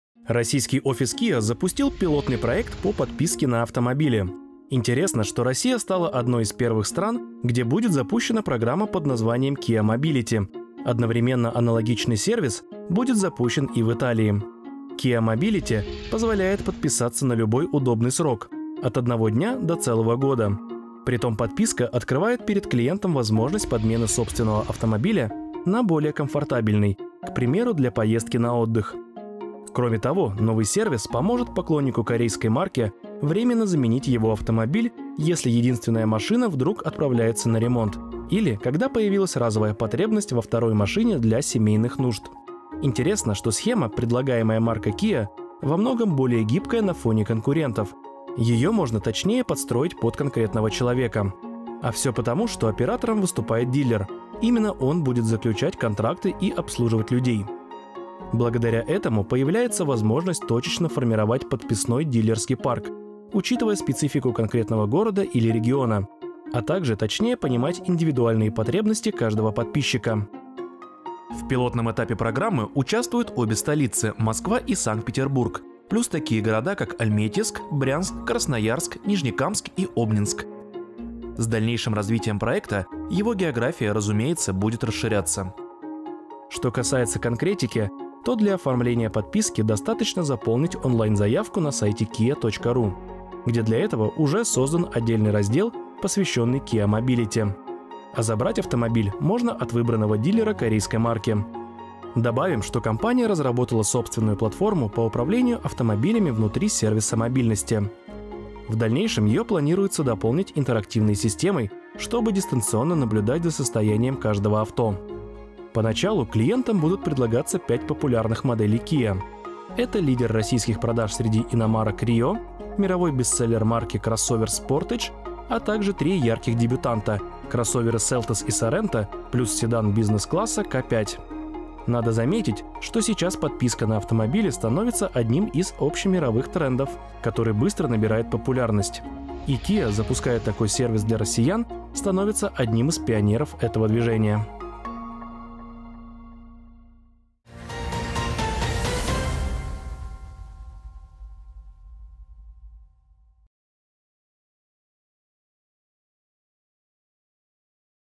KiaMobility открыла новую мобильность для фанатов Kia | Новости с колёс №1138
Интересно, что Россия стала одной из первых стран, где будет запущена программа под названием KiaMobility, — одновременно аналогичный сервис будет запущен и в Италии. Кроме того, новый сервис поможет поклоннику корейской марки временно заменить его автомобиль, если единственная машина вдруг отправляется на ремонт, или, когда появилась разовая потребность во второй машине для семейных нужд.